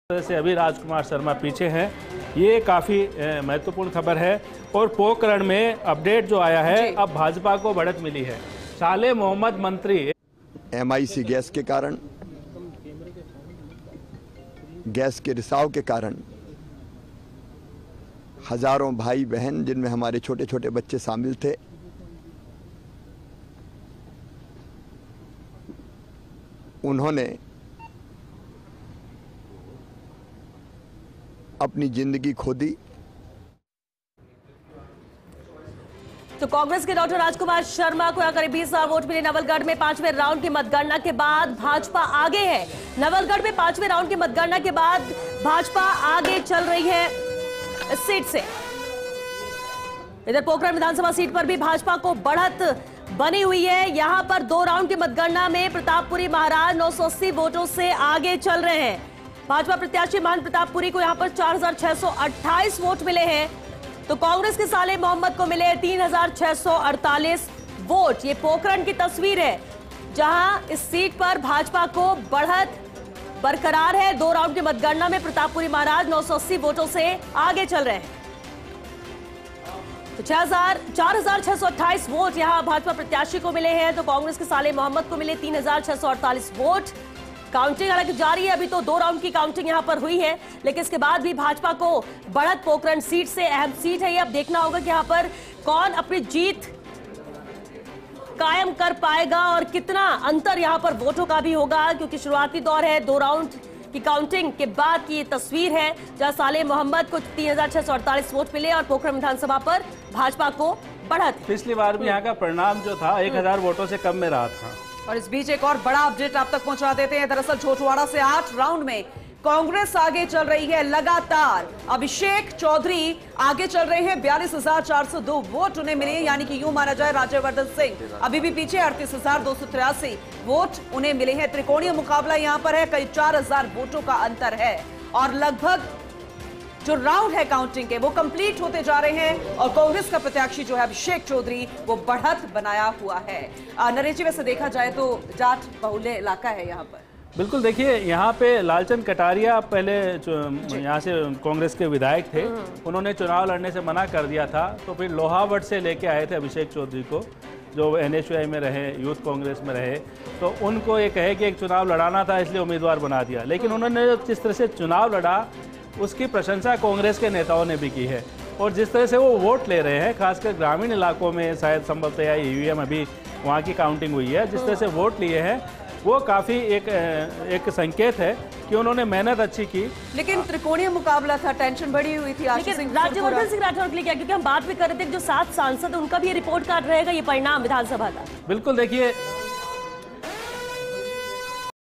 झोटवाड़ा विधानसभा में 8 वें राउंड में कांग्रेस आगे | Rajasthan Assembly Election Result 2023
About this Video:

Rajasthan Results Live: The Rajasthan state Assembly elections were held in a single phase on November 25. Of the 52.6 million registered voters in Rajasthan, 74.13 per cent cast their ballots to decide the fate of 1,875 candidates in the state. Rajasthan elections have been dominated by the Bharatiya Janata Party (BJP) and Congress, with the two parties alternating governments since 1993. In the 2018 Assembly elections, Congress secured 100 seats, one short of the majority, and formed the government with the support of Bahujan Samaj Party (BSP) and independents. BJP secured 70 seats, far less than the last elections, where it won 163 seats. Following the elections, Congress leader Ashok Gehlot began his third term as chief minister of Rajasthan. For the 2023 polls, both Congress and BJP have stated that their chief ministerial candidate will be decided after the elections.